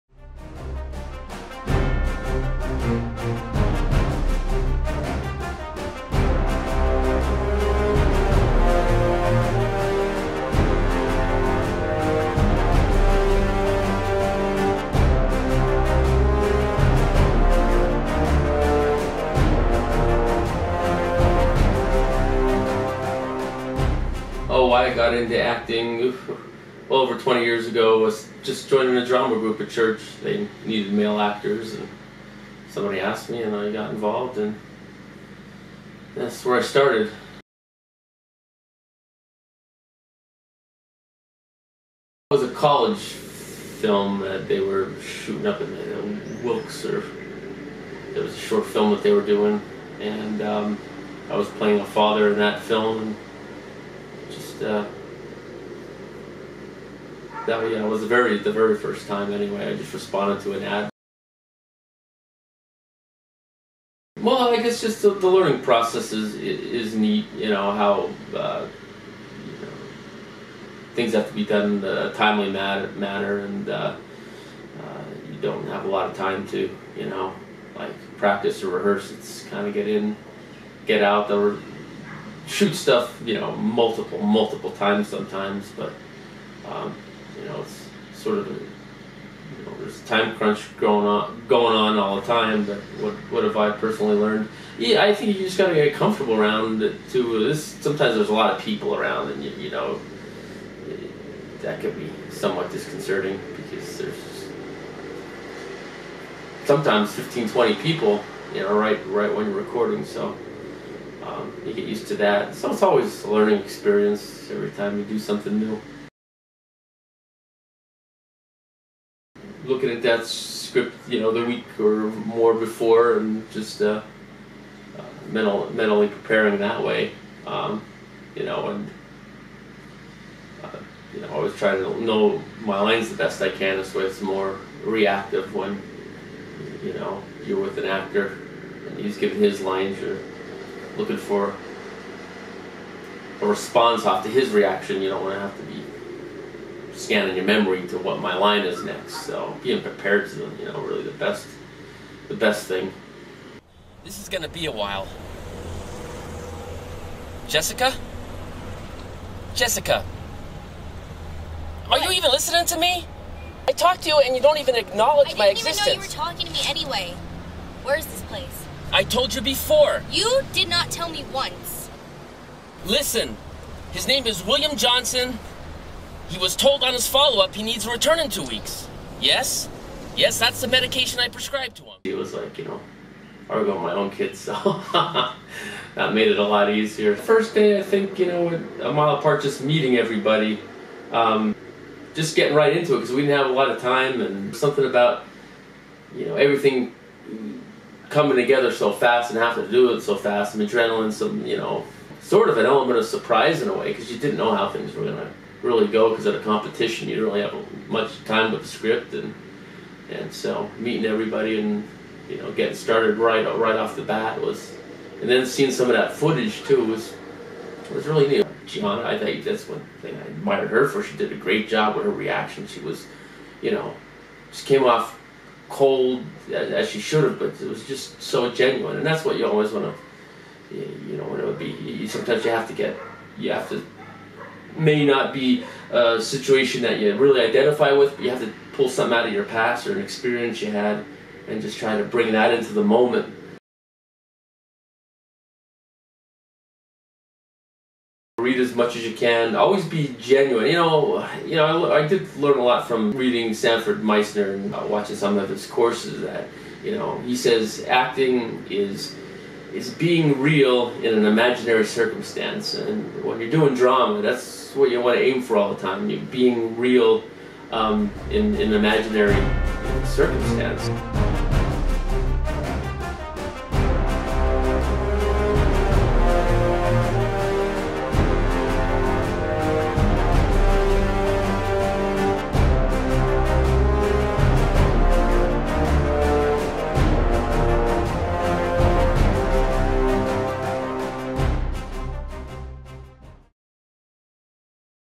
Steve Machita: Independent Acting (Directing the Future Episode 3)
We sit down with local actor, Steve Machita to talk about the start to his career in film, his television experience and about his newest film "A Mile Apart".